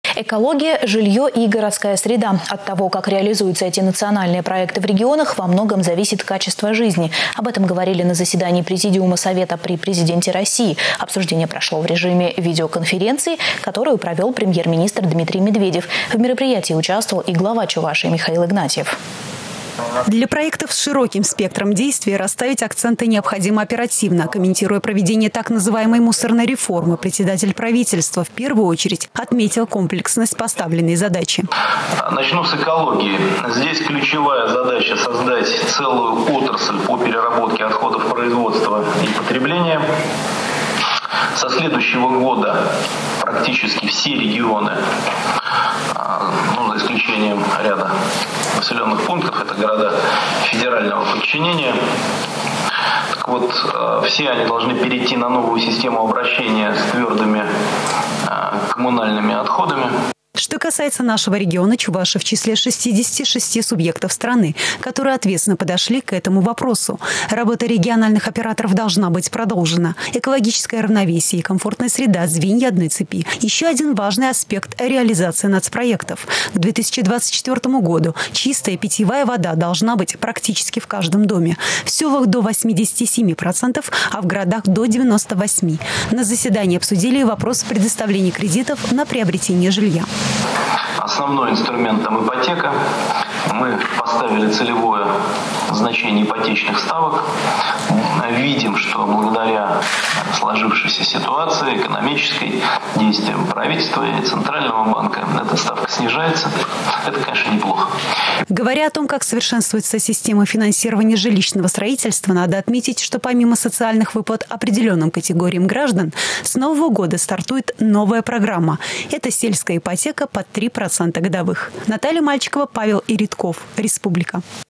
Прошло заседание президиума Совета при Президенте России
«Экология»,  «Жилье и городская среда» - от того как реализуются эти национальные проекты в регионах во многом зависит качество жизни. Об этом говорили на заседании президиума Совета при Президенте России. Обсуждение  прошло в режиме видеоконференции, которую провел премьер-министр Дмитрий Медведев. В мероприятии участвовал и Глава Чувашии Михаил Игнатьев.